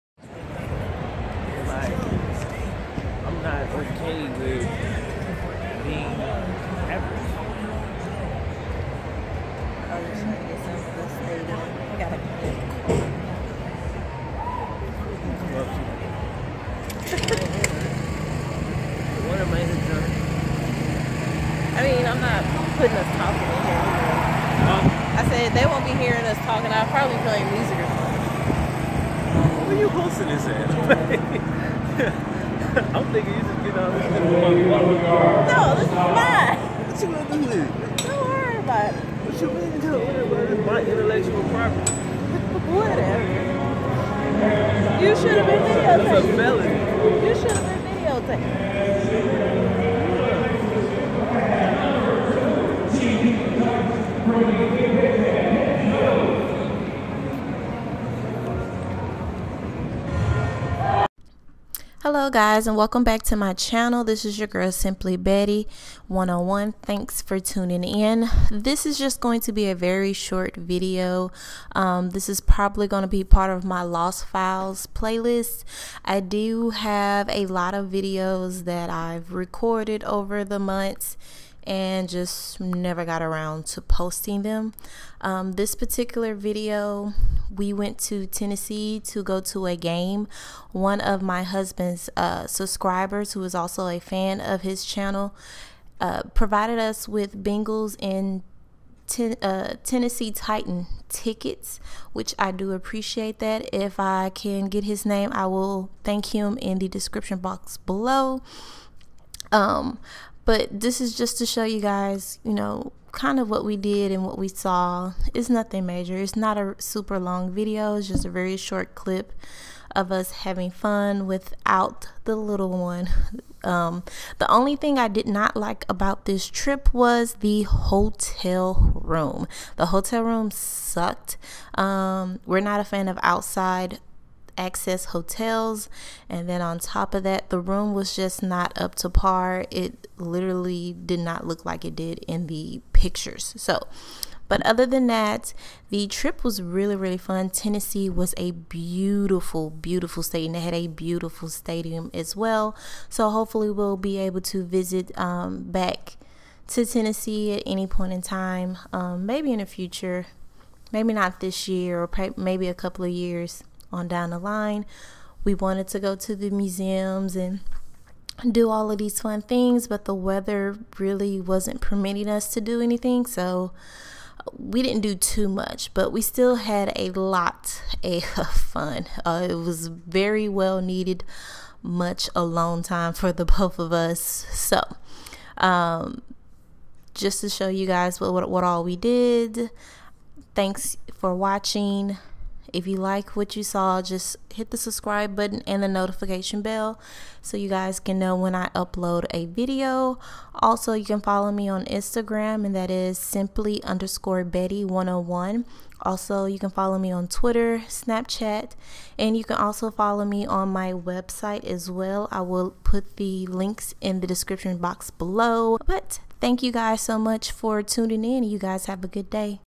Tennessee Trip Vlog: Much Needed
Big thanks to Willie Nelson for the Bengals vs Titans tickets

Thanks for tuning in, this is part of the lost files videos of a bunch of videos that I had been recorded.  Some were done even before I started my channel, so here is one of them.